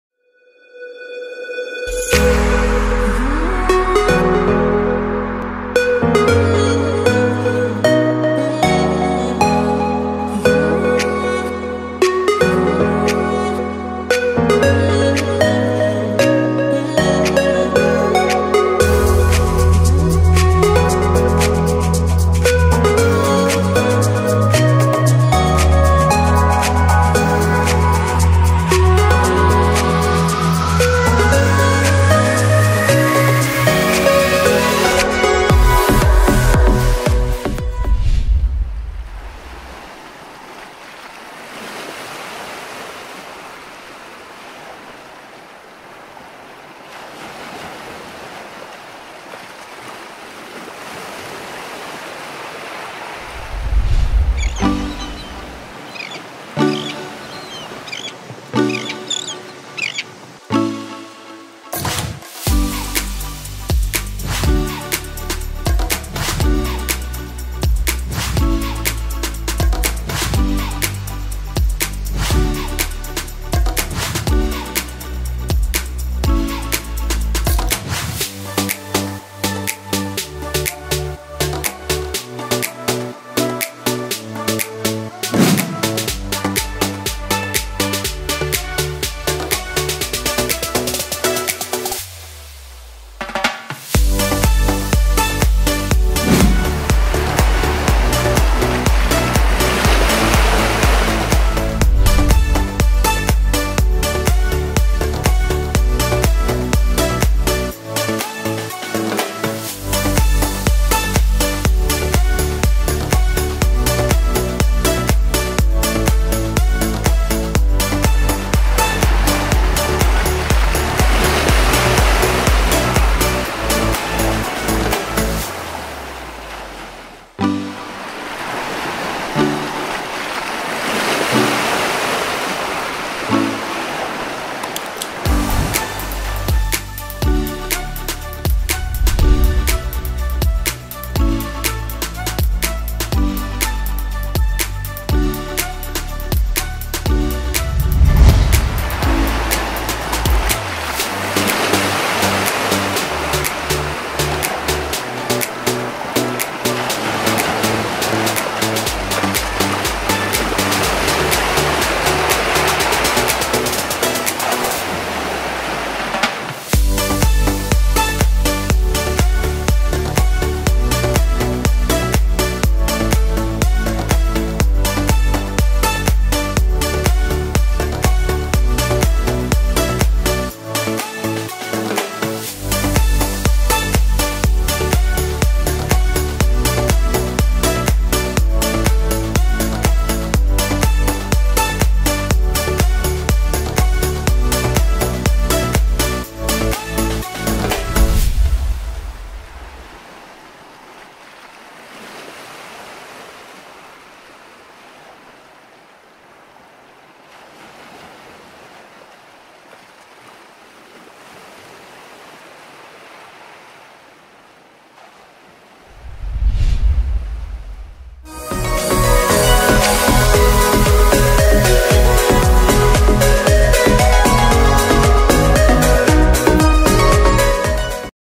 First Trip After Lockdown | Sattahip, Thailand
Sattahip, Thailand | Cinematic Travel

Nang Ram Beach | First trip after lockdown.